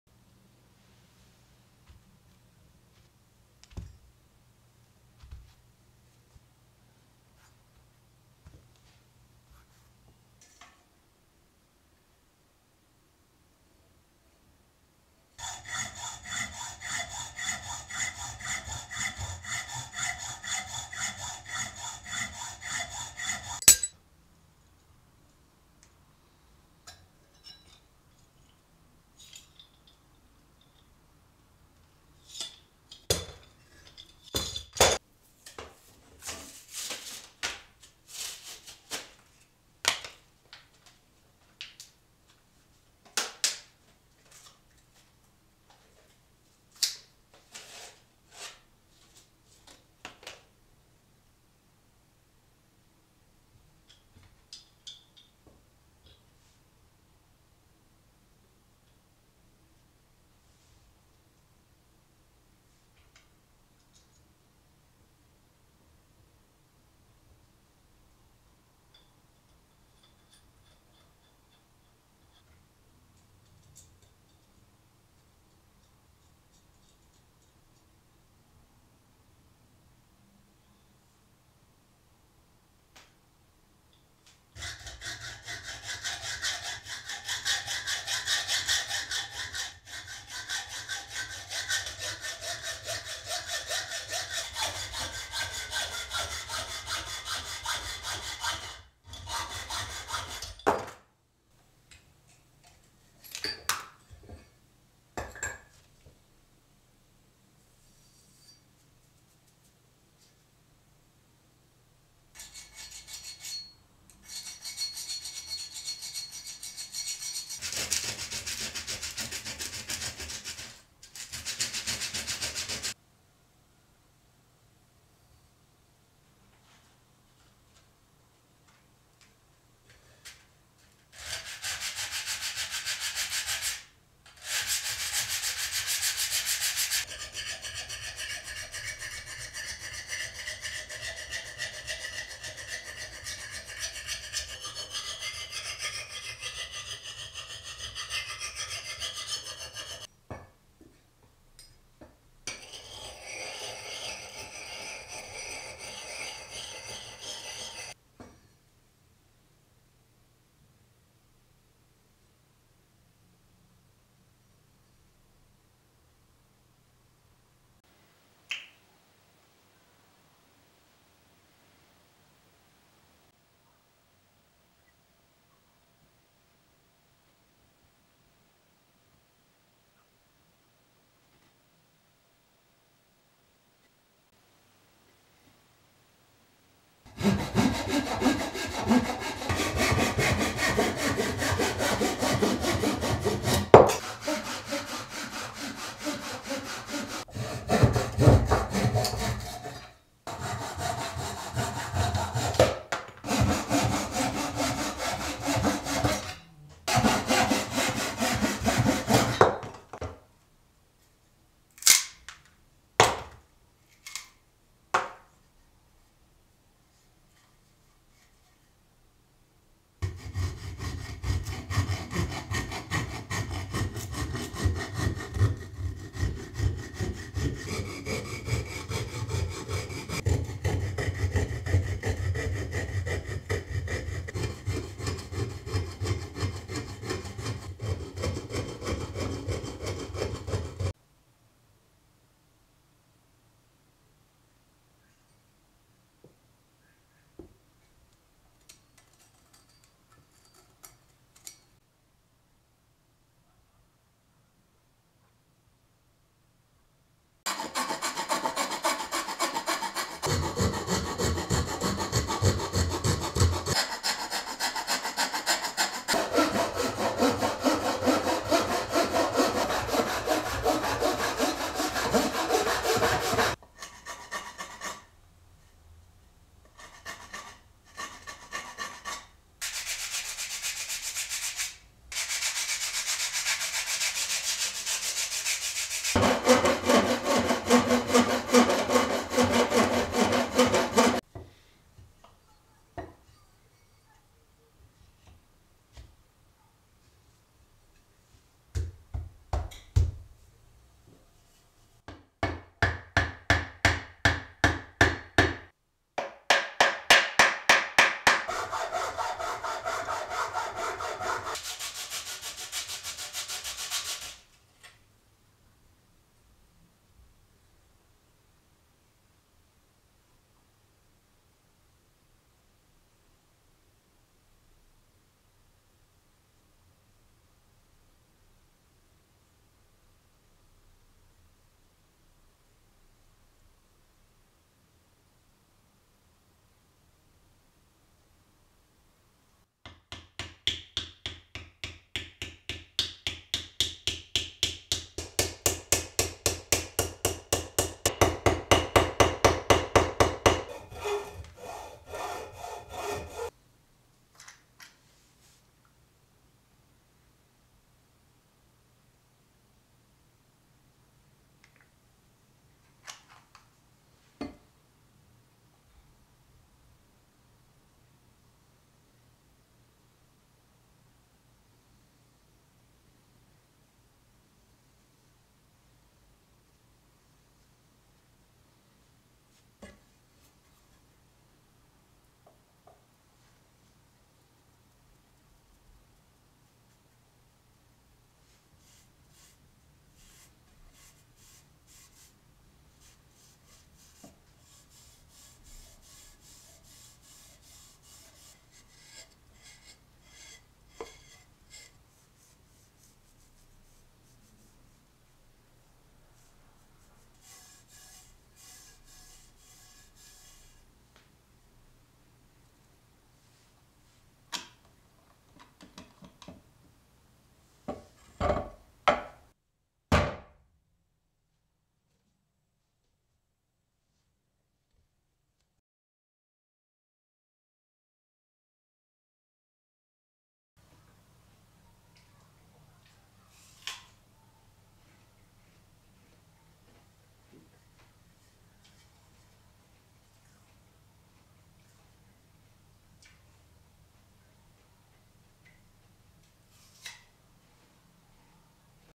Perfecting the axe | woodworking
Forming the desired shape, sharpening the blade and making a comfortable handle.
Note: I cut off part of the blade so that it would be more convenient and lighter to take this axe into hikes. This does not affect its functionality for my purposes.